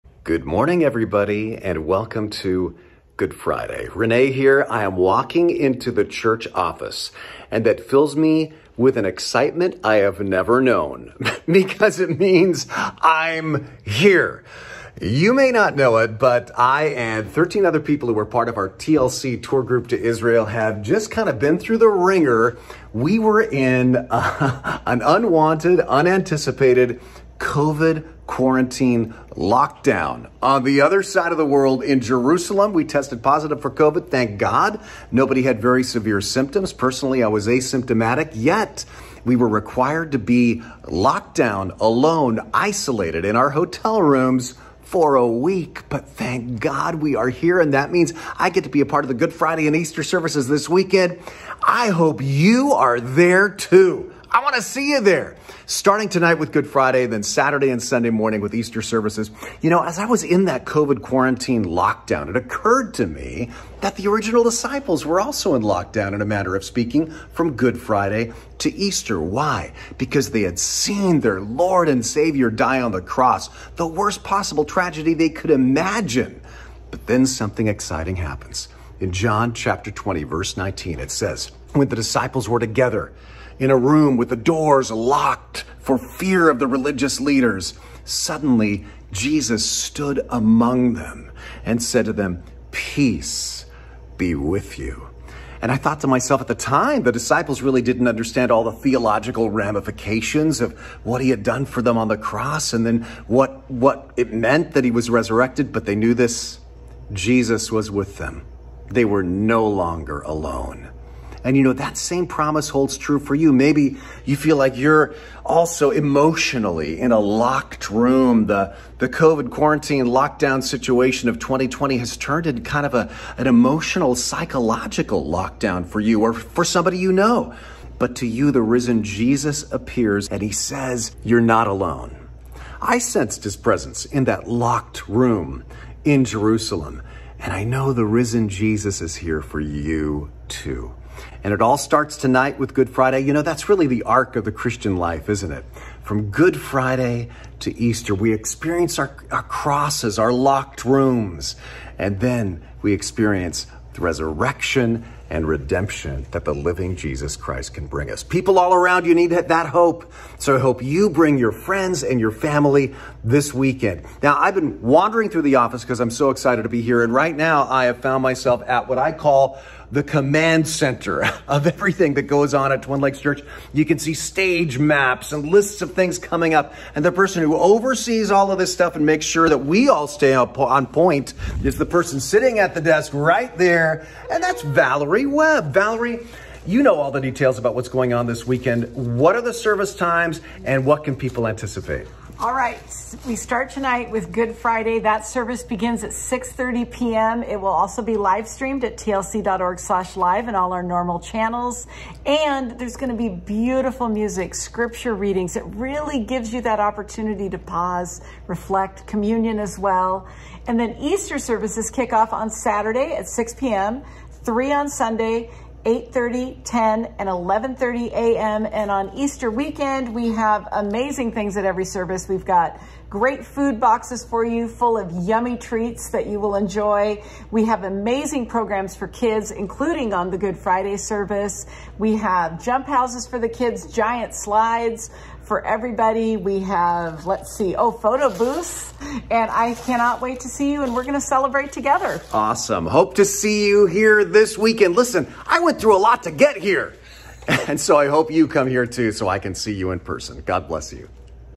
VIDEODEVO 2022 04 15
Someone is super happy to be back in the office today. Plus a preview of the weekend!